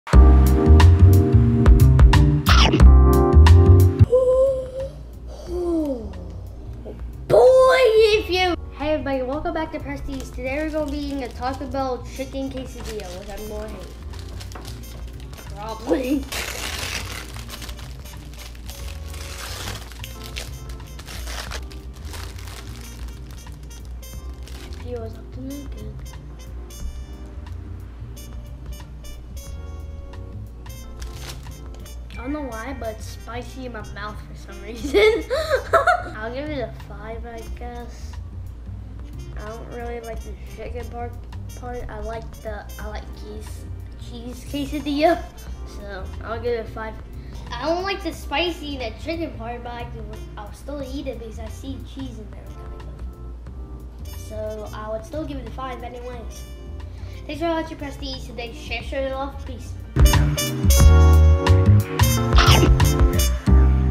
Preston Eats: Taco Bell Chicken Quesadilla
My dad switched the menu item on me at the last minute. I don't like chicken in my quesadillas! I'll give it a try...